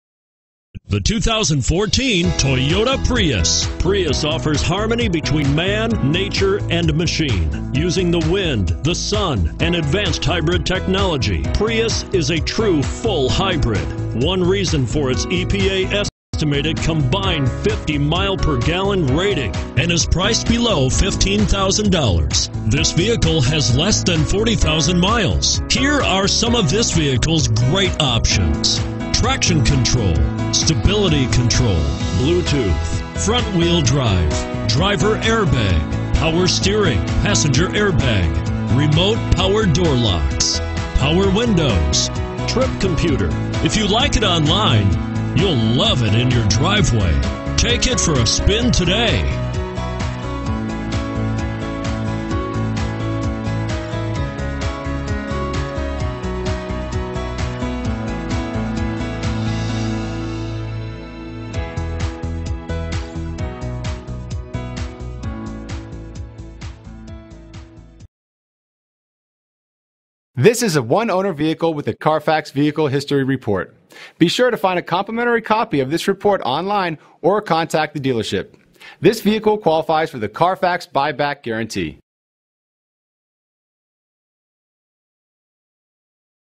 2014 Toyota Prius c DC, Maryland, and Virginia 95356
2014 Toyota Prius c ONE - $12495

You'll love this 2014 Toyota Prius c. This is a  you'll want to take home. With 38641 miles, it features Automatic transmission and an exterior color of Silver.